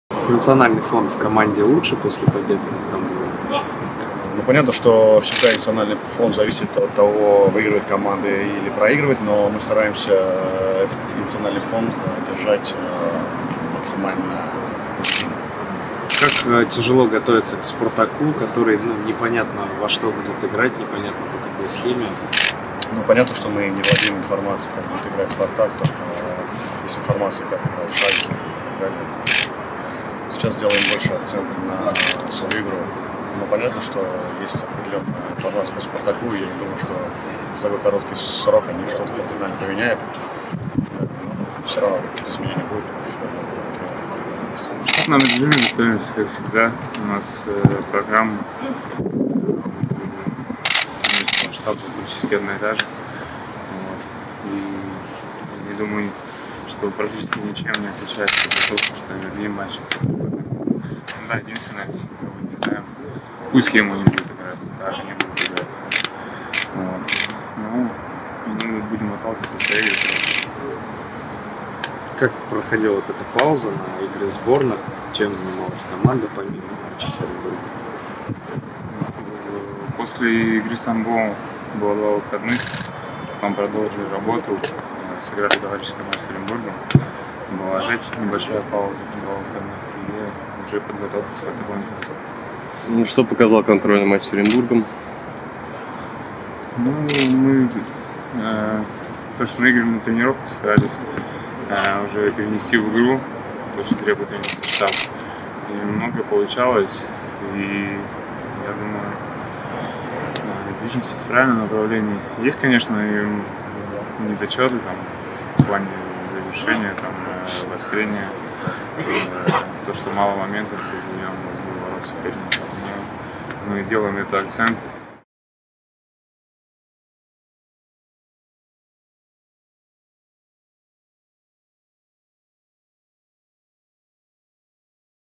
«Рубин» провел открытую тренировку перед матчем со «Спартаком»
Казанский «Рубин» в субботу на выезде сыграет со «Спартаком». В преддверии игры на базе казанского клуба прошла открытая тренировка.
Видео: Динар Ахметзянов